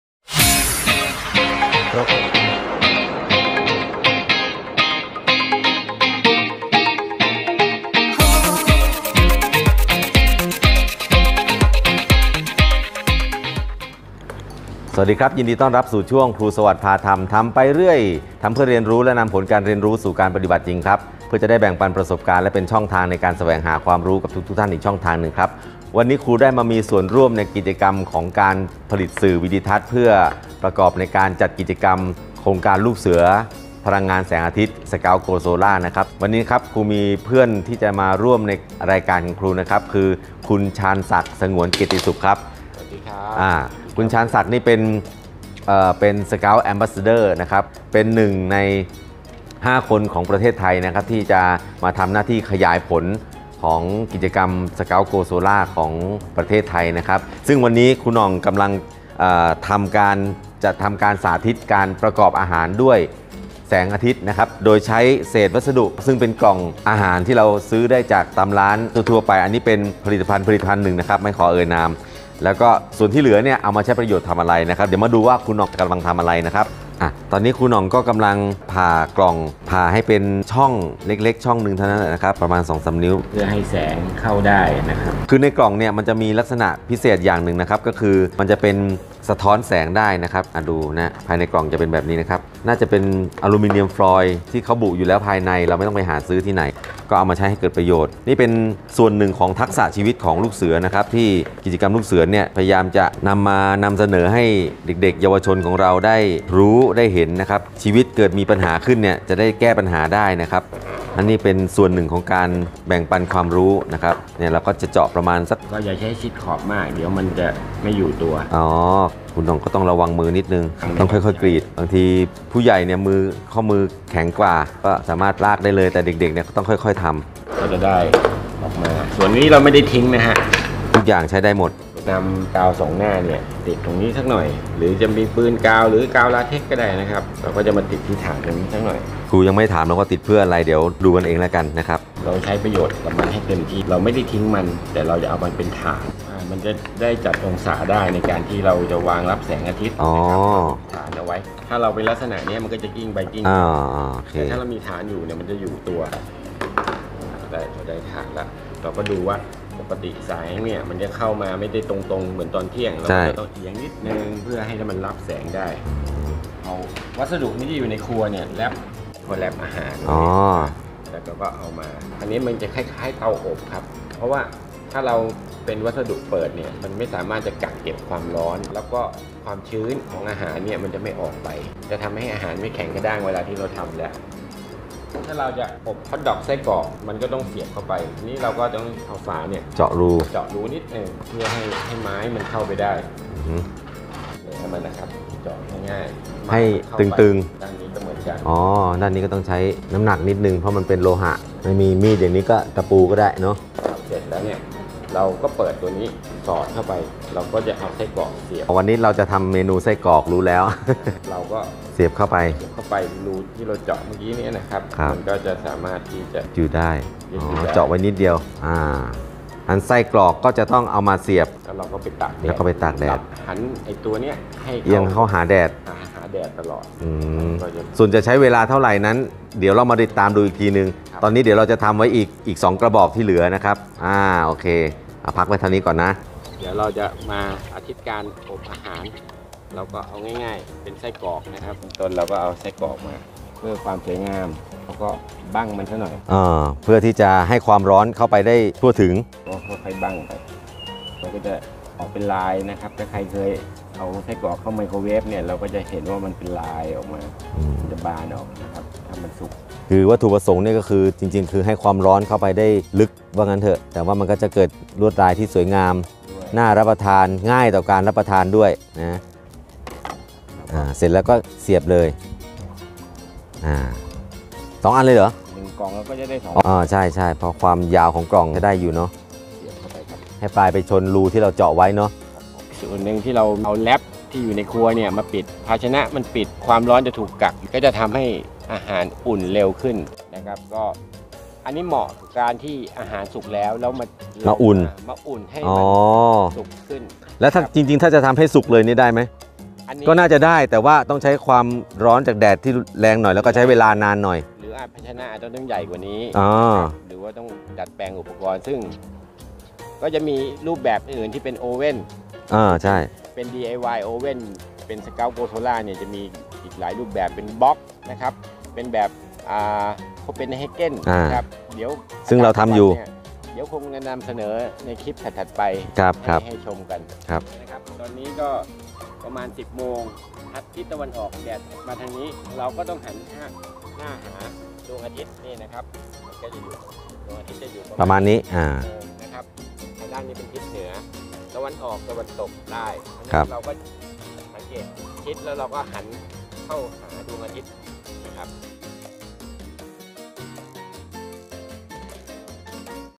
วิธีทำตู้อบพลังงานแสงอาทิตย์ จากกระบอกมันฝรั่ง
วิธีทำตู้อบฮอทดอก จากกระบอกมันฝรั่ง

การอบอาหารจากเตาอบโดยอาศัยพลังงานไฟฟ้า อาจถูกเปลี่ยนแนวคิด เมื่อได้เห็นตัวอย่างการประดิษฐ์วัสดุจากกระบอกมันฝรั่ง ให้เป็นตู้อบอาหาร โดยใช้พลังงานแสงอาทิตย์ จากโครงการ Scouts go Solar